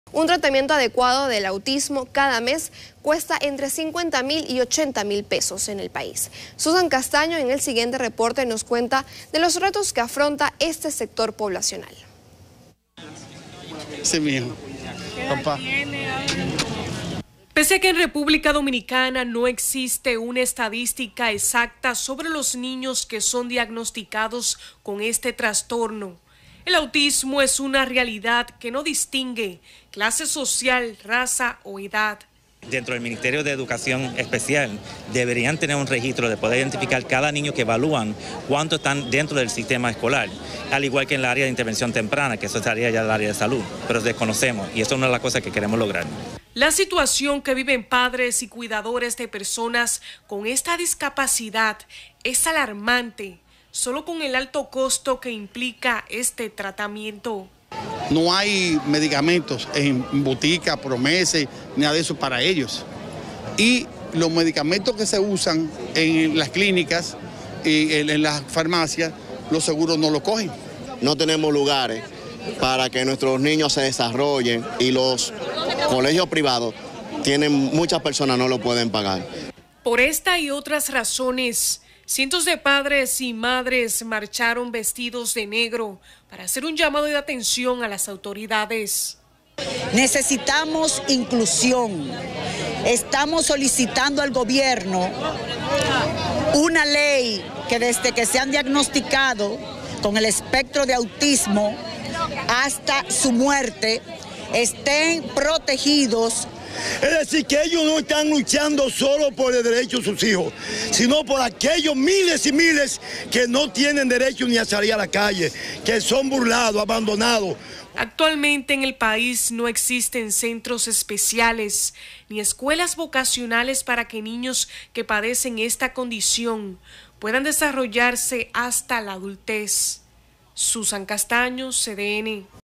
Un tratamiento de autismo en RD cada mes ronda entre los 50 a 80 mil pesos
Un tratamiento adecuado de autismo cada mes cuesta entre 50 a 80 mil pesos en el país.

Pese a que en República Dominicana no existe una estadística exacta sobre los niños que son diagnosticados con ese trastorno, el autismo es una realidad que no distingue clase social, raza o edad.